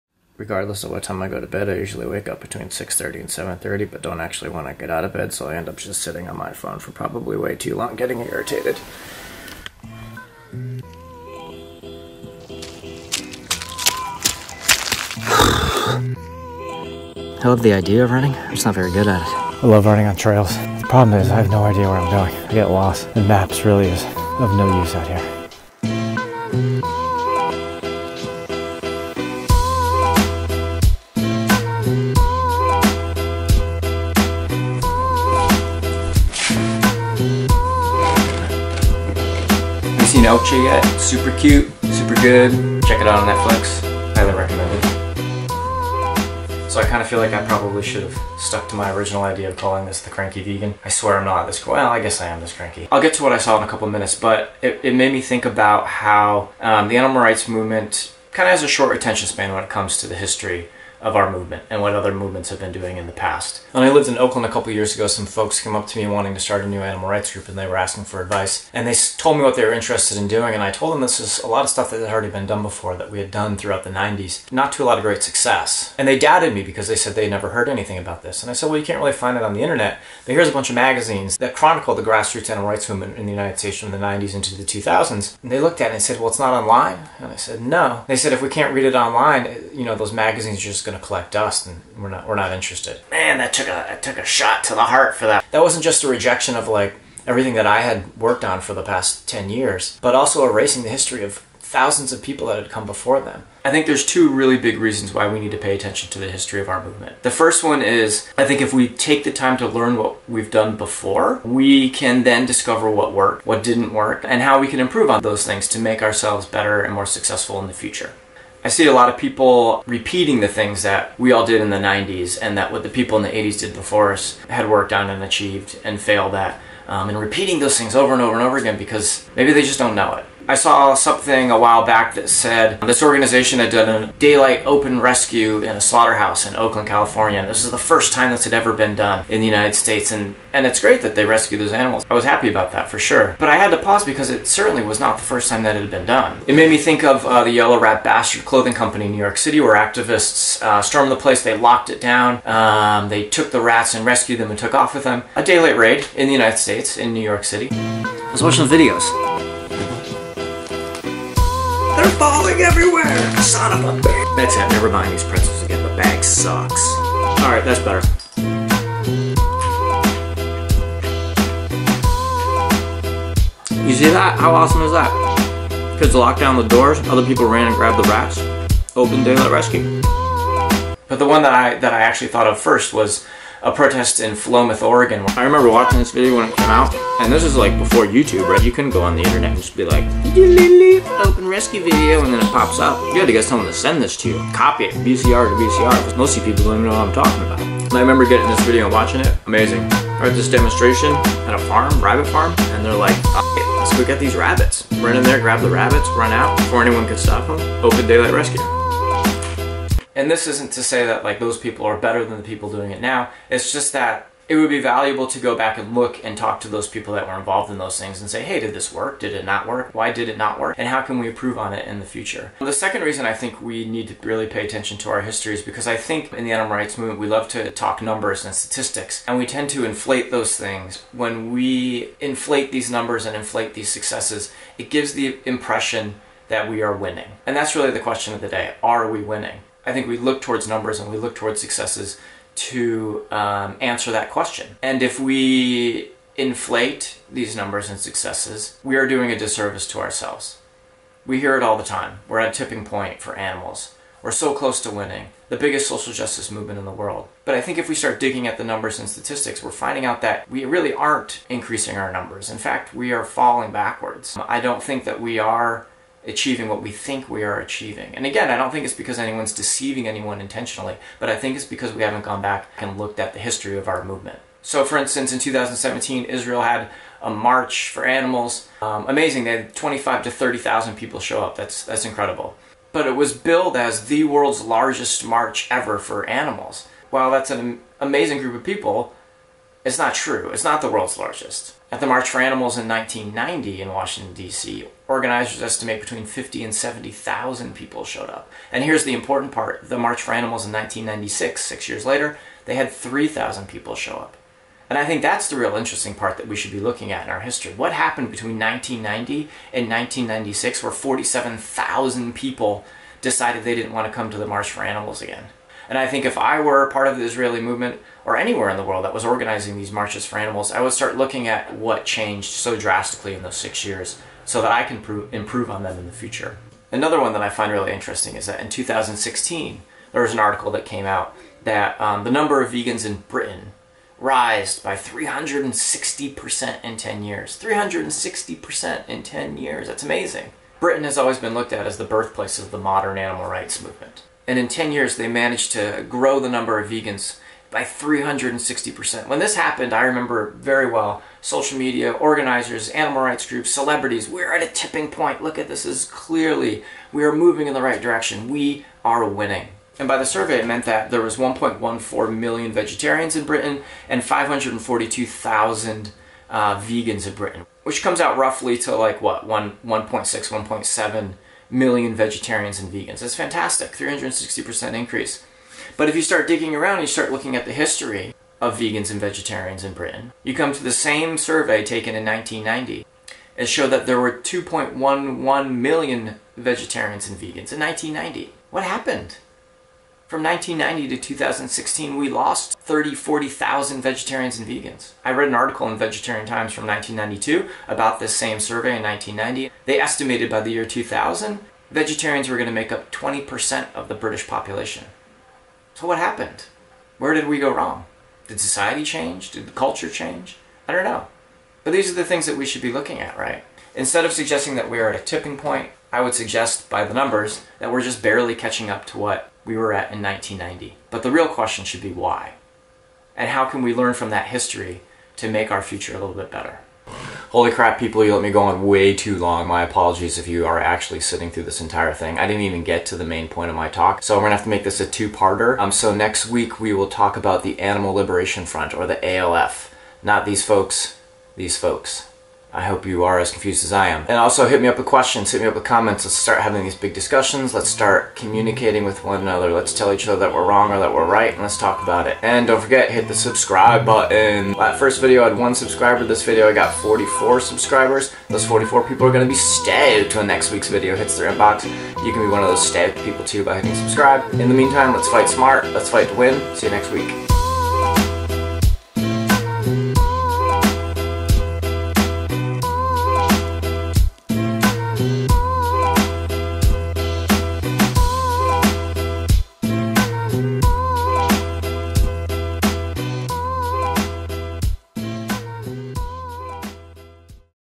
Are We Winning? // Know Your History, Part 1
¡Ahora puedes activar los subtítulos en español! Haz click en Configuración.

Is the animal rights movement winning when it overlooks it's rich history of tactics, strategies, successes & failures?